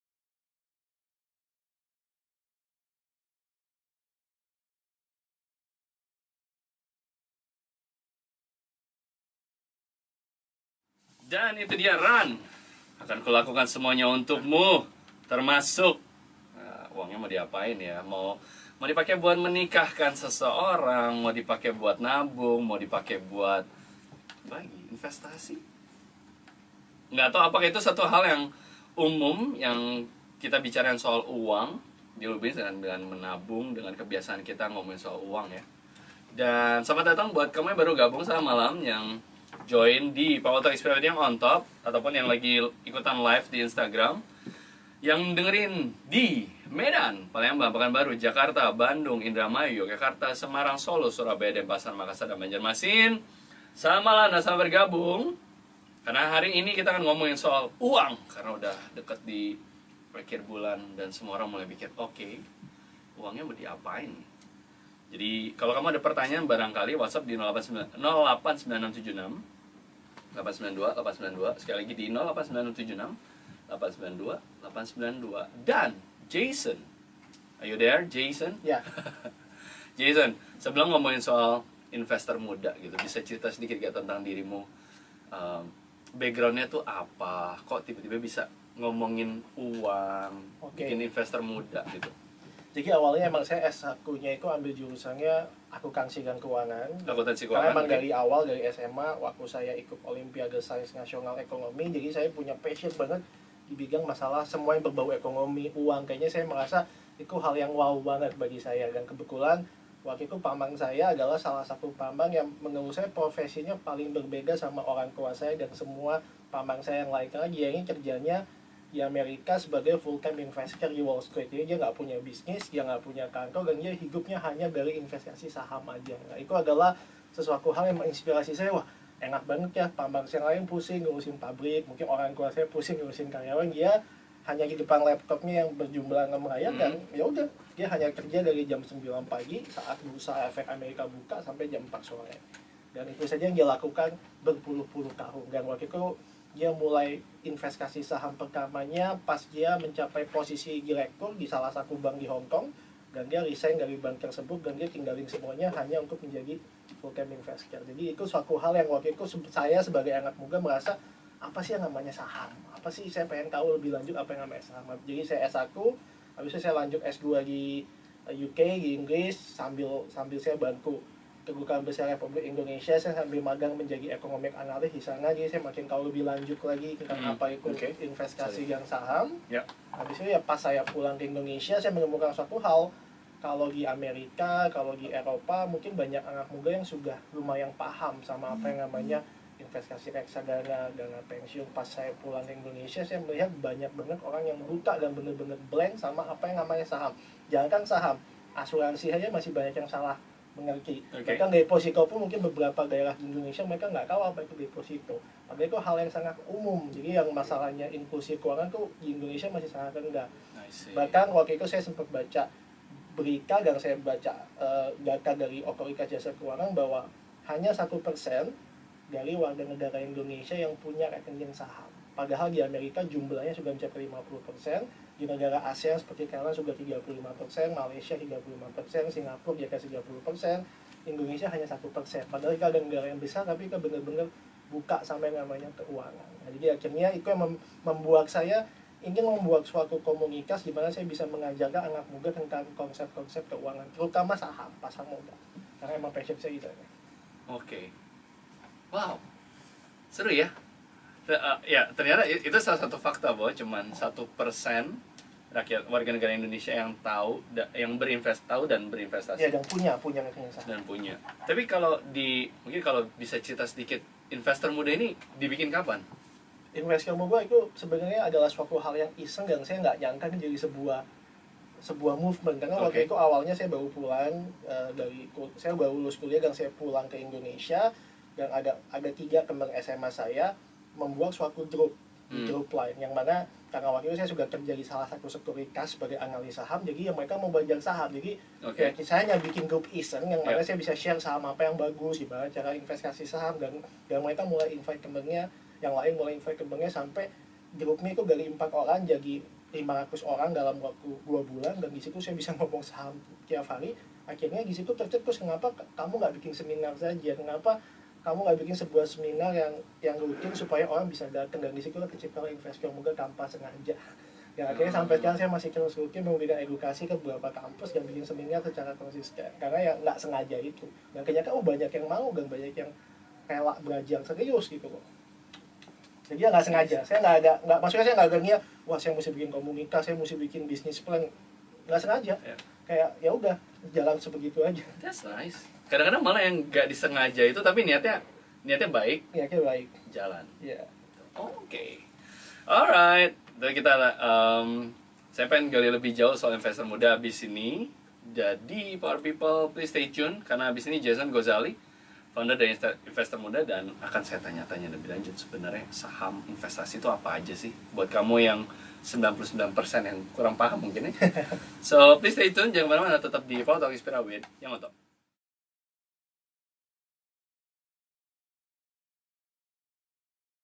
Jason Gozali: Inklusif Keuangan di Indonesia Masih Sangat Rendah
Narasumber InspiraWithYOT: Jason Gozali, Founder of Investor Muda